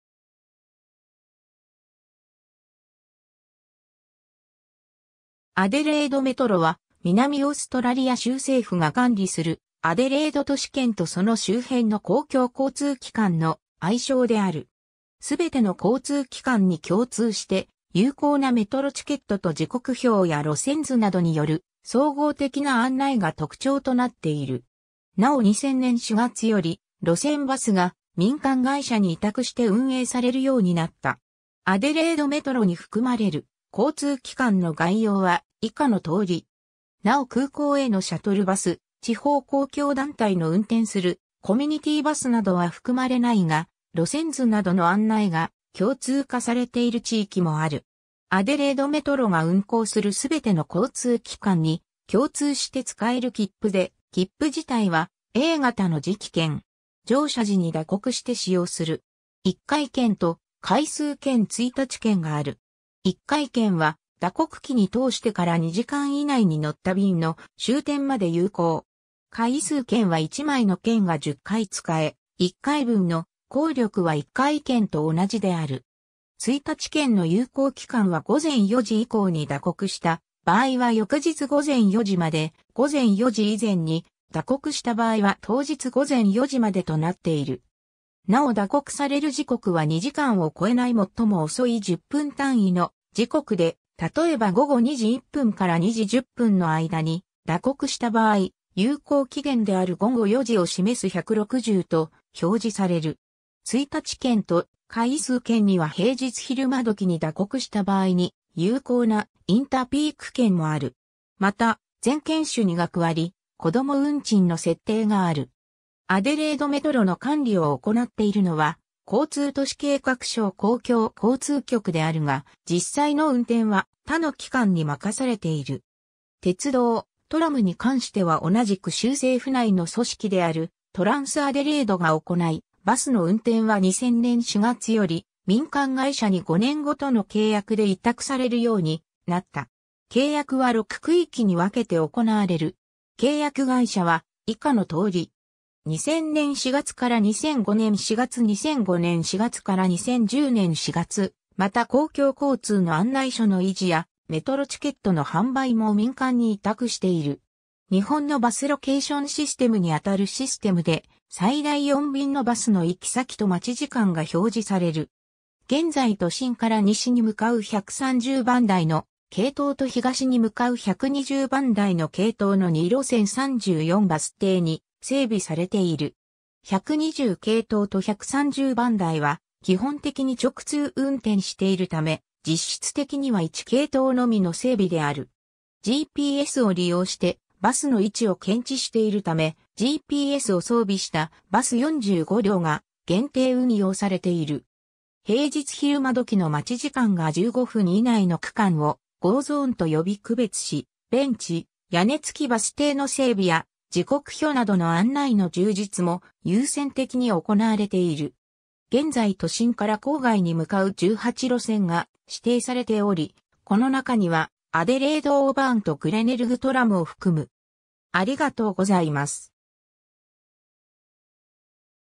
アデレード・メトロ
アデレード・メトロ (Adelaide Metro) は南オーストラリア州政府が管理するアデレード都市圏とその周辺の公共交通機関の愛称である。
全ての交通機関に共通して有効なメトロチケットと時刻表や路線図などによる総合的な案内が特徴となっている。
なお2000年（平成12年）4月より路線バスが民間会社に委託して運営されるようになった。
 アデレード・メトロに含まれる交通機関の概要は以下の通り。
 なお空港へのシャトルバス、地方公共団体の運転するコミュニティーバスなどは含まれないが、路線図などの案内が共通化されている地域もある。
 アデレード・メトロが運行する全ての交通機関に共通して使える切符で、切符自体はA型 (3.0cm * 5.75cm) の磁気券。
乗車時に打刻して使用する。
1回券 (Singletrip Ticket) と回数券 (Multitrip Ticket) 、1日券 (Daytrip Ticket) がある。
1回券は打刻機に通してから2時間以内に乗った便の終点まで有効。
回数券は1枚の券が10回使え、1回分の効力は1回券と同じである。
1日券の有効期間は午前4時以降に打刻した場合は翌日午前4時まで、午前4時以前に打刻した場合は当日午前4時までとなっている。
なお打刻される時刻は2時間を越えないもっとも遅い10分単位の時刻で、例えば午後2時1分から2時10分の間に打刻した場合、有効期限である午後4時を示す160と表示される。
1日券と回数券には平日昼間時（午前9時1分から午後3時まで）に打刻した場合に有効な、インターピーク (Interpeak) 券もある。
また全券種に学割・子供運賃の設定がある。
 アデレード・メトロの管理を行っているのは交通都市計画省 (Department of Transport and Urban Planning) 公共交通局 (Office of Public Transport) であるが、実際の運転は他の機関に任されている。
鉄道・トラムに関しては同じく州政府内の組織であるトランスアデレード (TransAdelaide) が行い、バスの運転は2000年4月より民間会社に5年ごとの契約で委託されるようになった。
契約は6区域（右図）に分けて行われる。
契約会社は以下の通り。
 2000年4月 — 2005年4月 2005年4月 — 2010年4月 (2005年2月に決定) また公共交通の案内所の維持やメトロチケットの販売も民間に委託している。
 日本のバスロケーションシステムにあたるシステムで、最大4便のバスの行き先と待ち時間が表示される。
現在都心から西に向かう130番台の系統 (Henley Beach Road) と東に向かう120番台の系統 (The Parade) の2路線34バス停に整備されている。
120系統と130番台は基本的に直通運転しているため、実質的には1系統のみの整備である。
GPSを利用してバスの位置を検知しているため、GPSを装備したバス45両が限定運用されている。
 平日昼間時の待ち時間が15分以内の区間をゴーゾーンとよび区別し、ベンチ・屋根つきバス停の整備や時刻表などの案内の充実も優先的に行われている。
現在都心から郊外に向かう18路線が指定されており、この中にはアデレード・オーバーンとグレネルグ・トラムを含む。